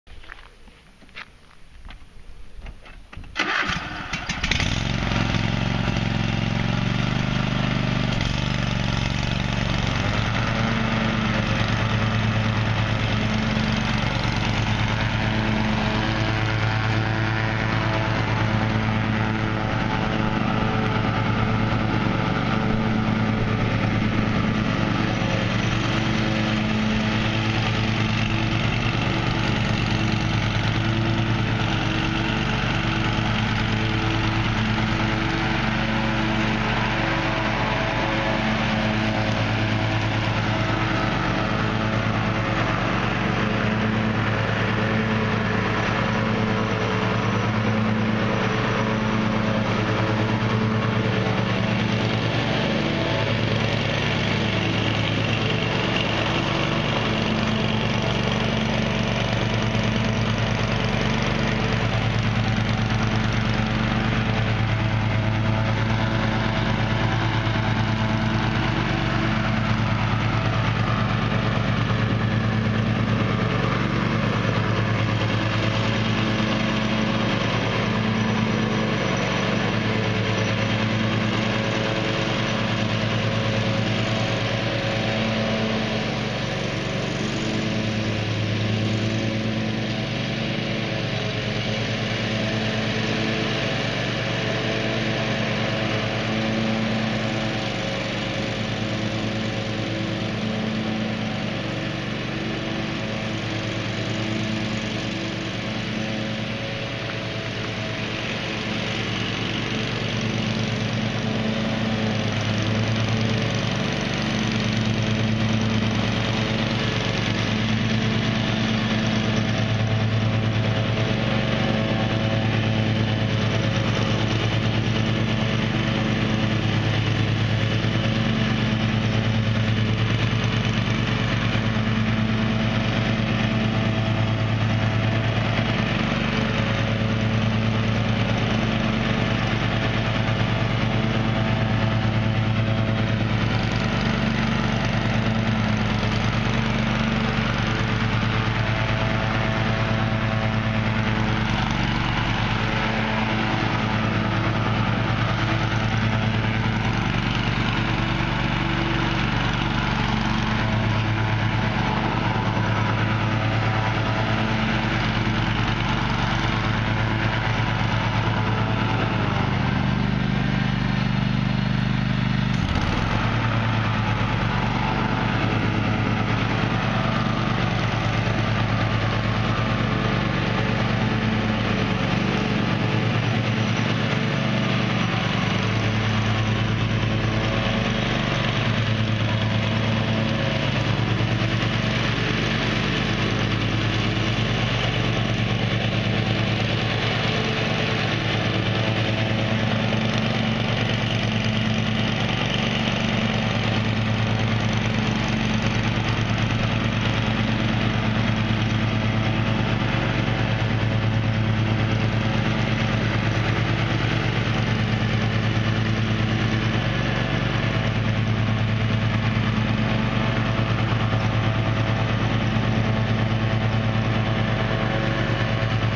Cub Cadet Zero Turn Rider
25 HP 50 inch deck.

 I used it for two years and sold it only because all of the steering linkage was getting worn out and it would of been a pain to replace. Also the linkage to the two transmissions was worn and I had to make custom tensioners, everything worked fine but top speed was maybe 1 or 2 mph slower. All of that stuff would of been need to be replaced within two or three seasons so I sold it while I was ahead. It did have 700 hours on it which is somewhat high. The motor has a lot of power @ 25 HP. It also had twin transmissions (one for each wheel), so straight forward it is true 2wd like a posi traction. My only major beef with the mower is it had a small fuel tank and it used way more gasoline than my 22hp 46" rider.